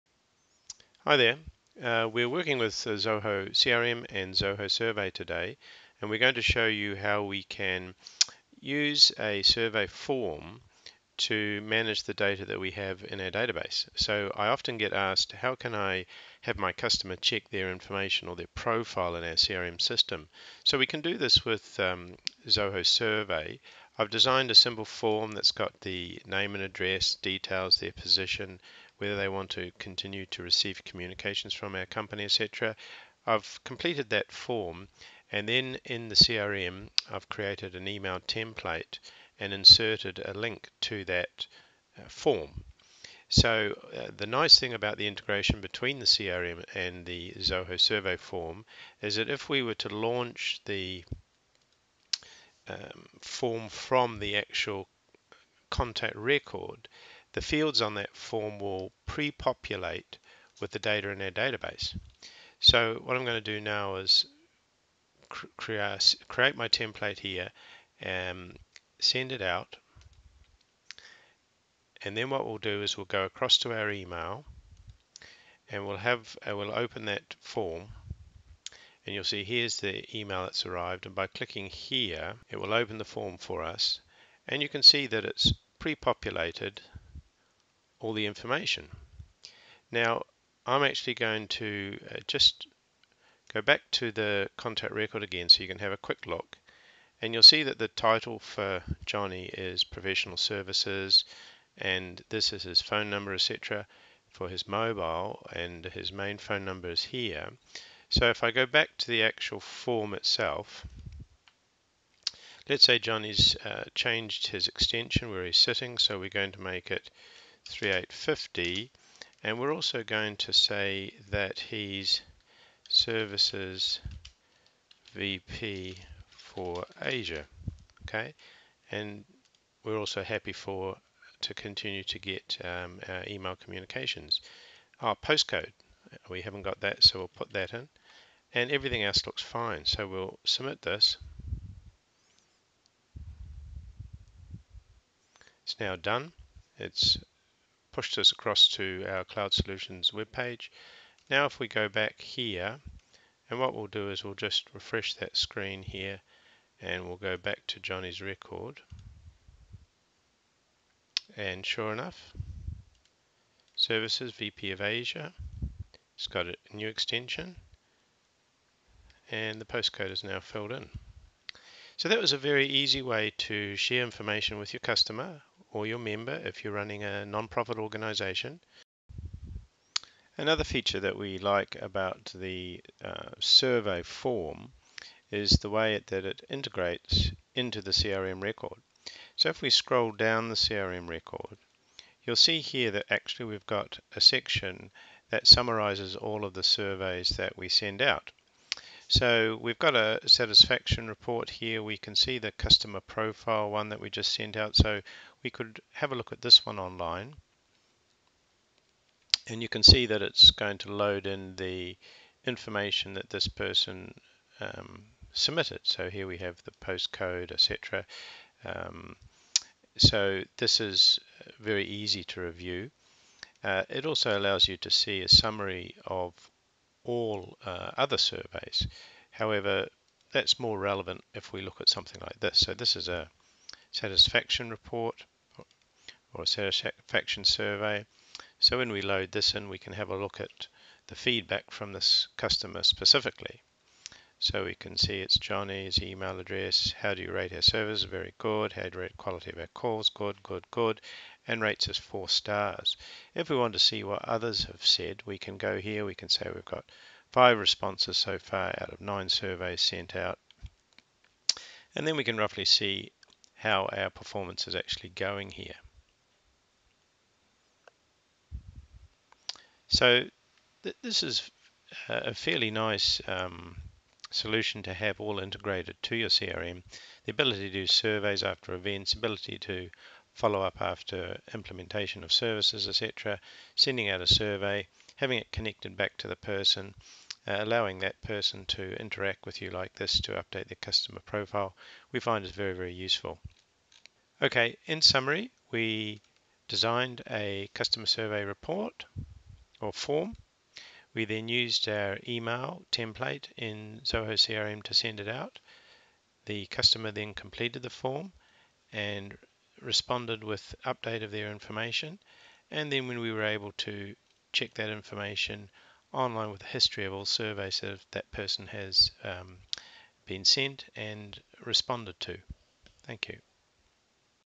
Zoho Survey and Zoho CRM
How to use Zoho Survey to allow your customers to review their data stored in your Zoho CRM system. Integration of Zoho Survey with CRM for customer satisfaction reporting. Contact Cloud Solutions for more information.